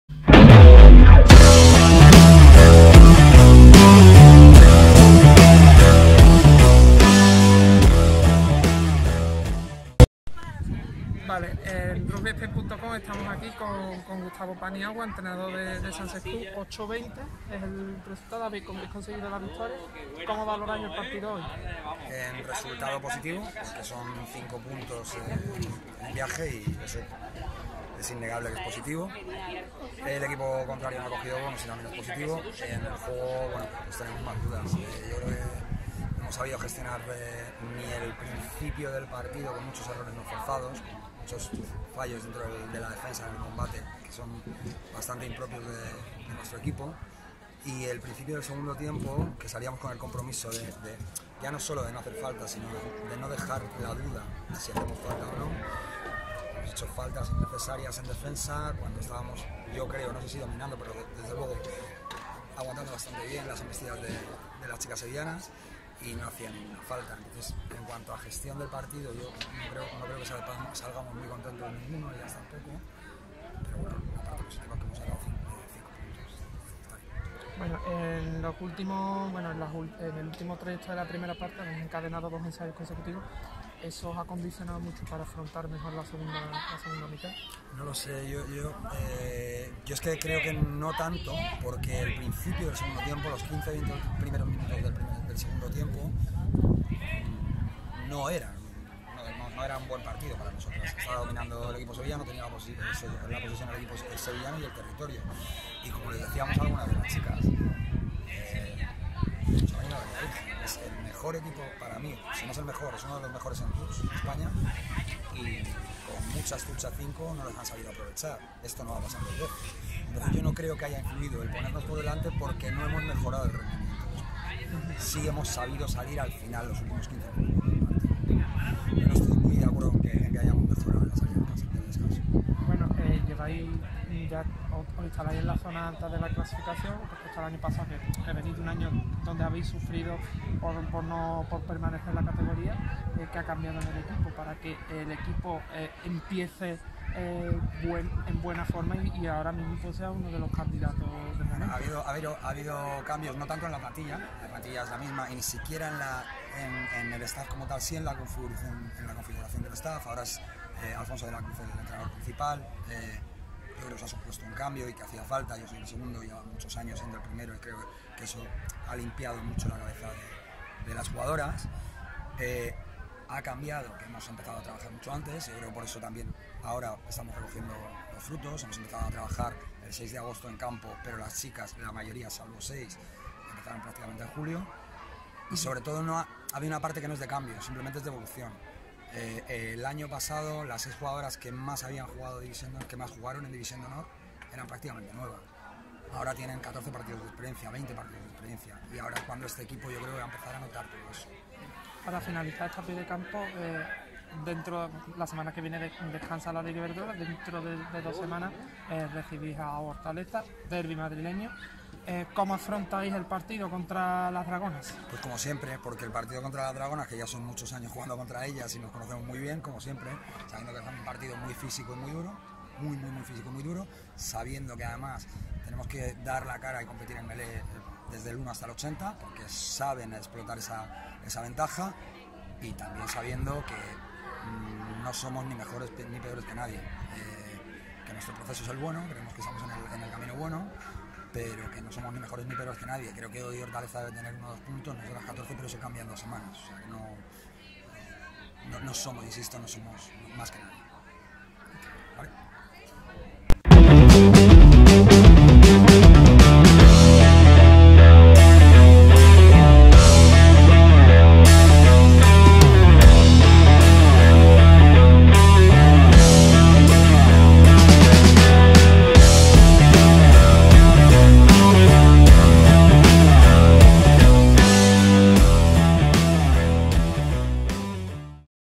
Entrevista Gustavo Andrés Paniagua (Sanse Scrum) | RugbySpain TV
Jesús León entrevista a pie de campo Entrenador Sanse Scrum, Gustavo Andrés Paniagua |